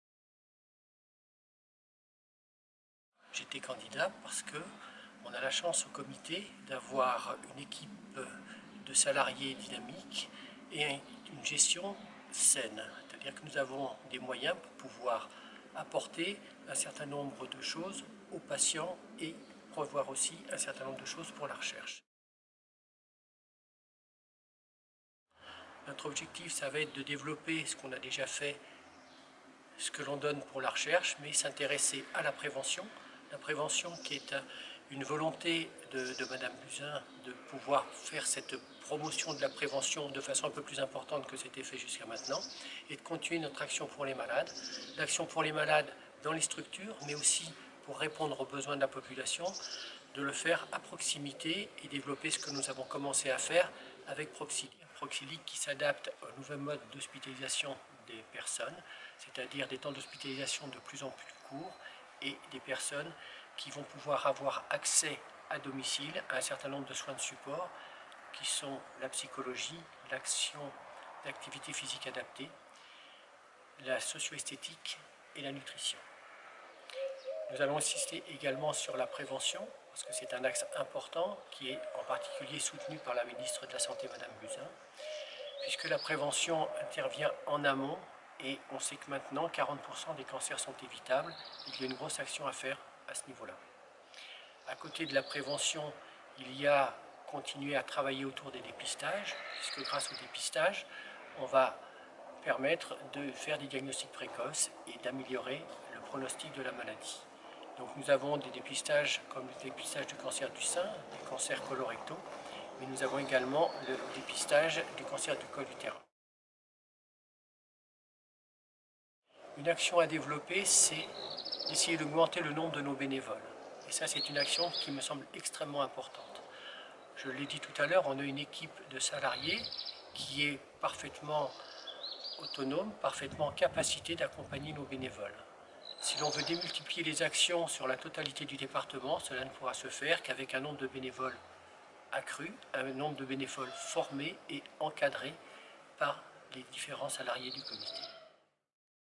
Le mot du nouveau Président de la Ligue contre le cancer de Loire-Atlantique
À l’occasion de l’Assemblée Générale de la Ligue contre le cancer de Loire-Atlantique, qui s’est tenue le mardi 17 avril 2018 à Nantes, le Docteur Philippe Bergerot a été élu président de l’association.

Il succède ainsi au Docteur Marc Grégoire qui a souhaité céder la présidence après 8 ans de fonction.